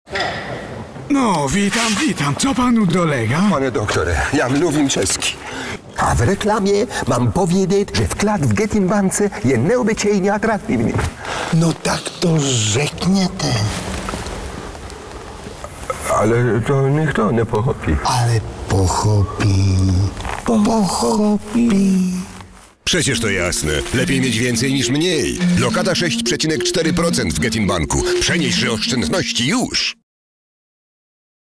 Getin Bank z udziałem Piotra Fronczewskiego: doktor [marketing-news.pl]
Kampania, zachęcająca Polaków do przeniesienia swoich oszczędności na lokatę 6,4%, rozpoczęła się w tym tygodniu i potrwa do końca czerwca. Na najnowszą odsłonę kampanii telewizyjnej z udziałem Piotra Fronczewskiego składają się dwa spoty: pt. „Czeski" oraz „Doktor".